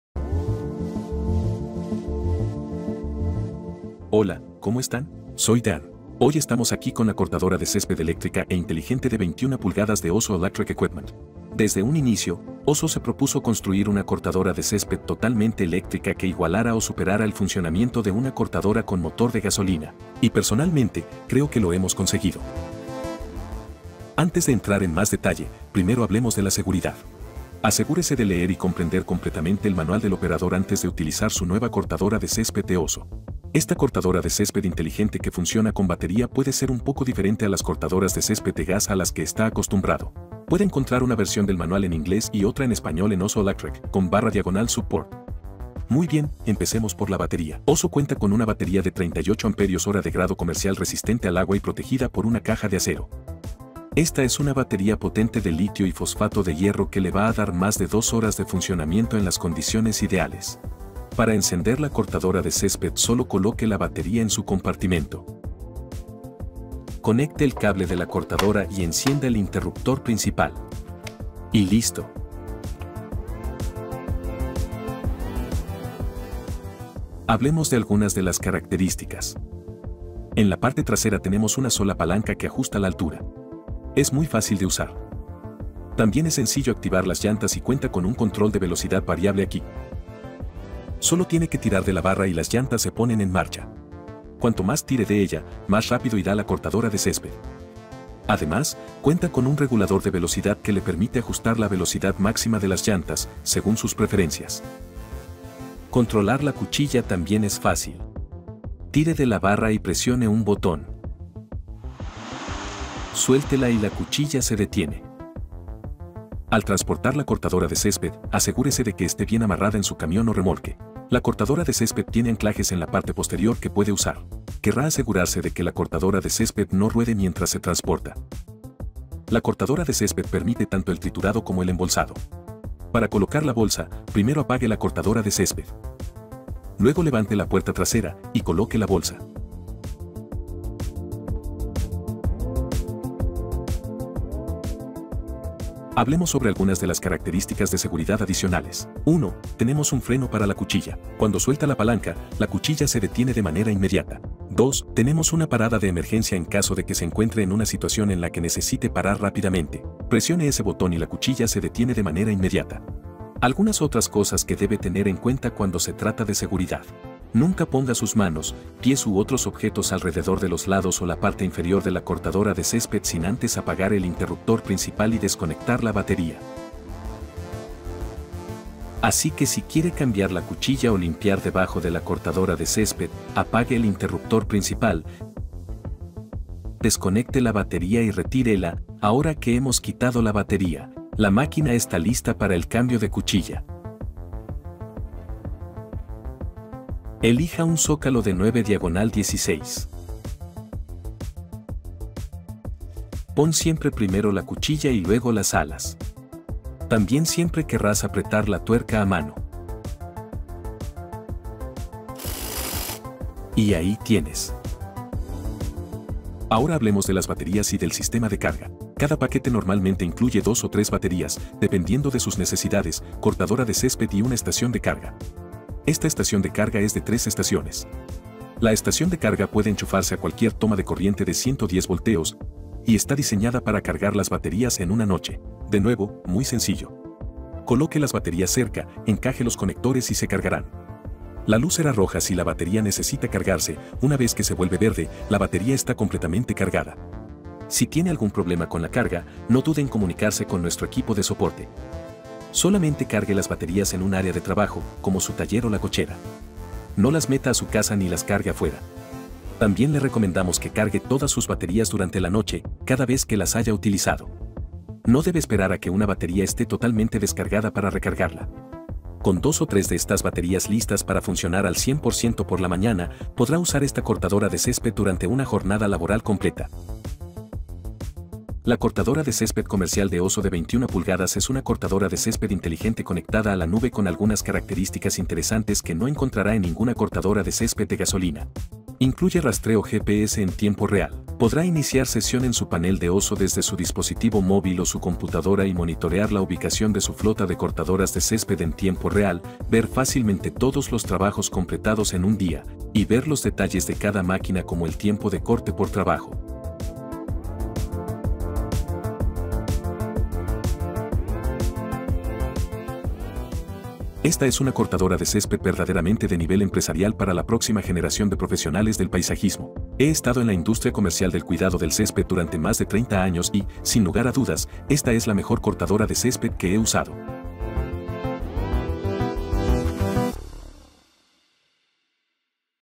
Cortadora de Césped Commercial Eléctrica de 21" de Oso Electric Equipment
Visita osoelectric.com para obtener más información. Oso Electric Equipment ha desarrollado la cortadora de césped eléctrica comercial de última generación de 21 pulgadas con una duración de batería de 2 horas por carga. Sumergámonos en detalle en cómo usar la cortadora de césped con este video.